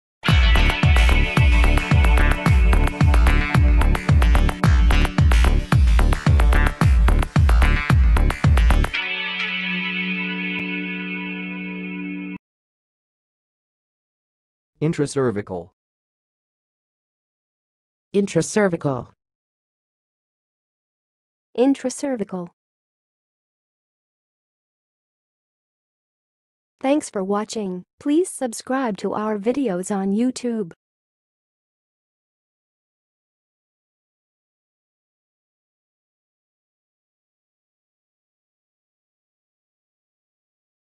How to say "intracervical"! (High Quality Voices)
Watch in this video how to say and pronounce "intracervical"!